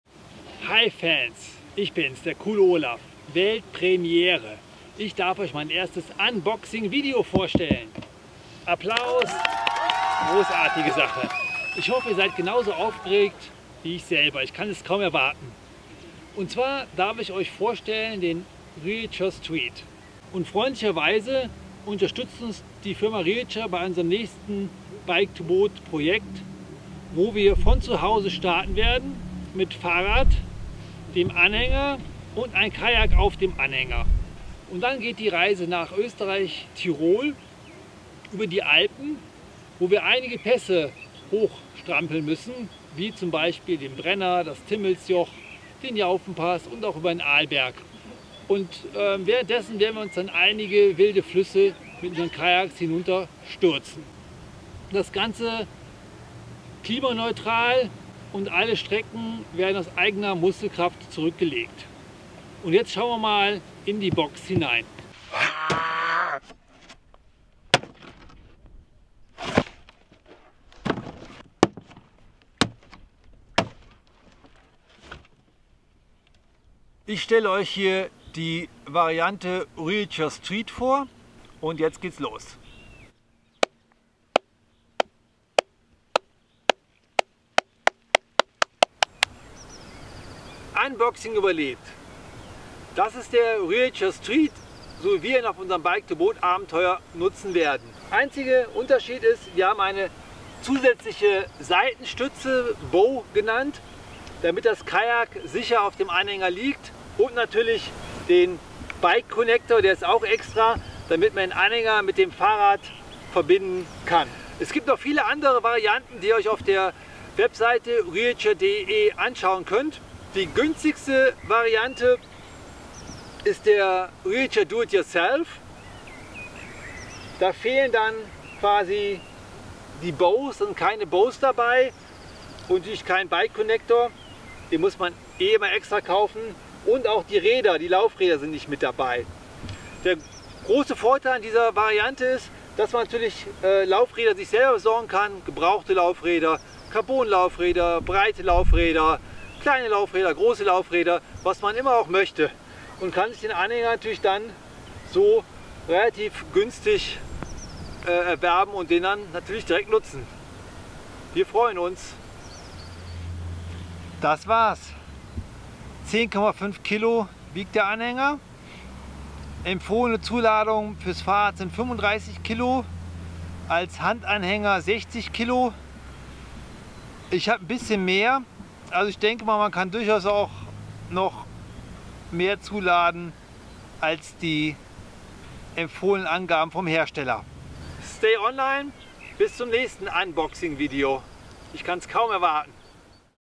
Unboxing reacha street - Fahrradanhänger für Kajaks.
Bei unseren Bike2Boat Abenteuern werden wir von der Firma reacha unterstützt. Der reacha street ist das perfekte Produkt, um sein Kajak von A nach B zu transportieren.